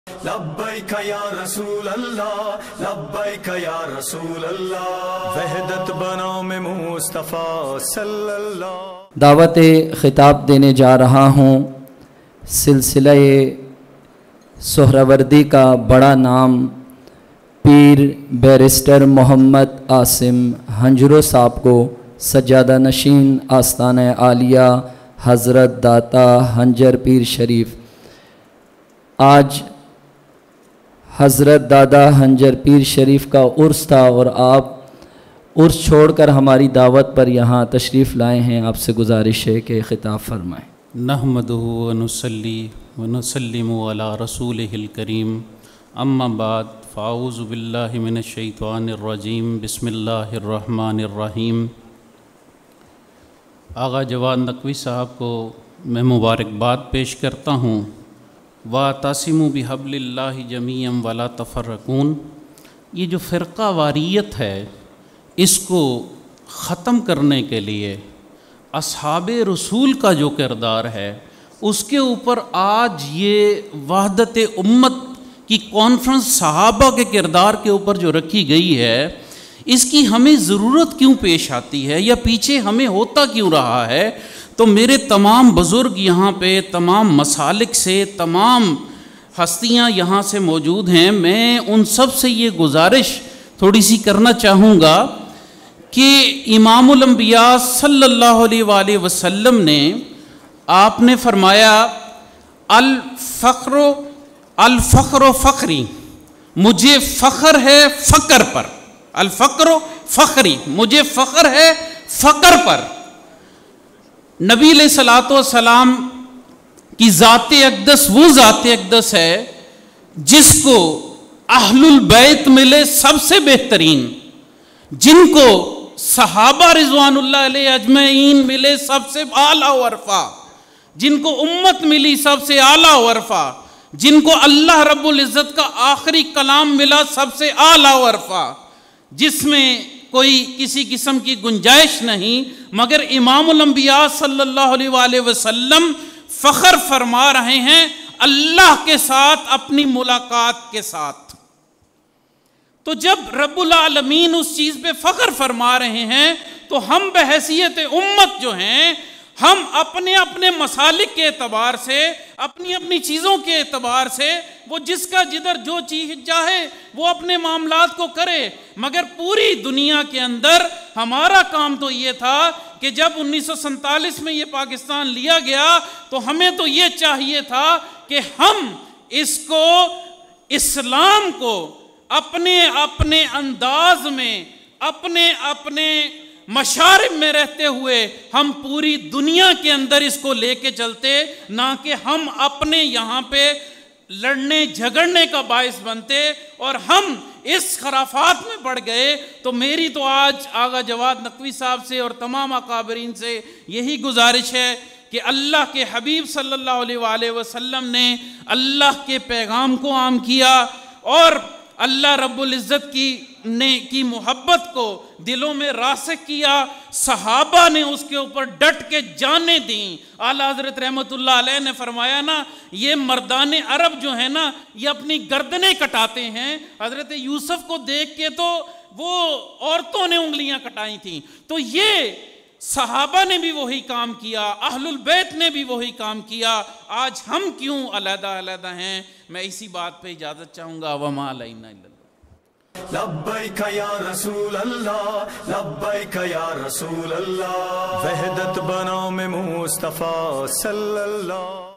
جناب پیر محمد عاصم ہنجرو صاحب || وحدت امت کانفرنس 2023
💚وحدت امت کانفرنس2023
بعنوان: وحدت امت میں اصحابِ رسول اللہ ص کا کردار
🗓️ یکم اکتوبر 2023
🕌 بمقام: مسجد بیت العتیق ،جامعہ عروۃ الوثقی' لاہور